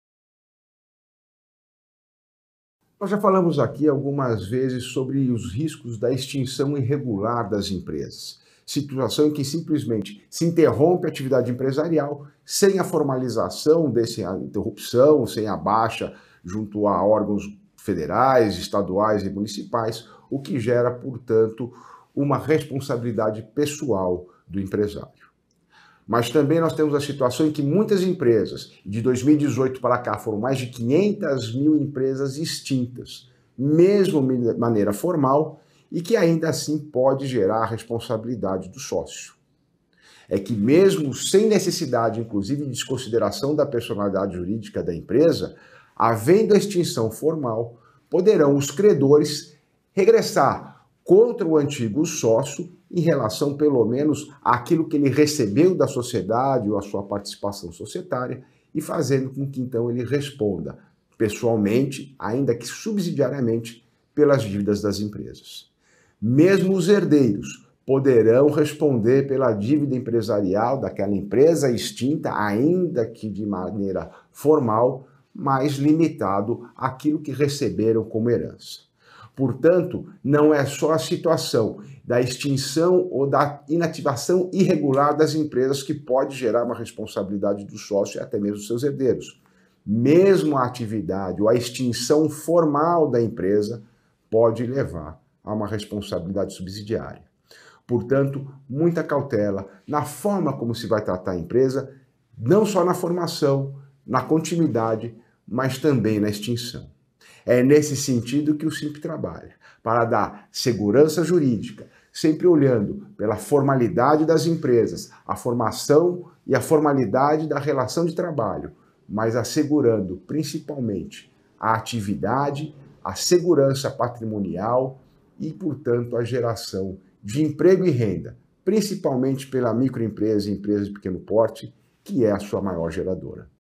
Consequências do encerramento irregular das empresas
A extinção irregular das empresas ocorre quando se interrompe a atividade empresarial sem formalização, interrupção ou a baixa junto aos órgãos federais, estaduais e municipais, o que gera, uma responsabilidade pessoal do empresário.
Contudo, desde 2018 mais de 500.000 empresas foram extintas de maneira formal, mas ainda assim pode gerar responsabilidade do sócio e desconsideração da personalidade jurídica da empresa.  Havendo a extinção formal, poderão os credores regressar contra o antigo sócio em relação a sua participação societária, fazendo com que responda pessoalmente, ainda que subsidiariamente, pelas dívidas das empresas. Mesmo os herdeiros poderão responder pela dívida empresarial daquela empresa extinta, ainda que de maneira formal, mas limitado àquilo que receberam como herança. “Muita cautela na forma como se vai tratar a empresa, não só na formação, na continuidade, mas também na extinção”, alerta Dr. Marcos Tavares Leite, advogado.
SINDICATO DA MICRO E PEQUENA INDUSTRIA DE RONDÔNIA